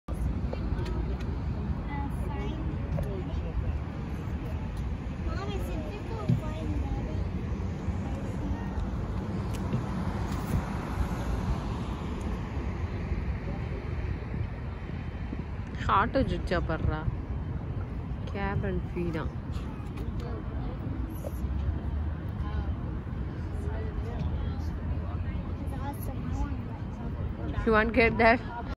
America vil auto rickshaw (அமெரிக்கா வில் ஆட்டோ ரிக்க்ஷா)
Chennai Ponnu in USA Vlogging begins! Ini than aarambam! America Auto rickshaw !Entertainment! Interesting facts we cross through in our life! Flawless nature!live the moments with peace!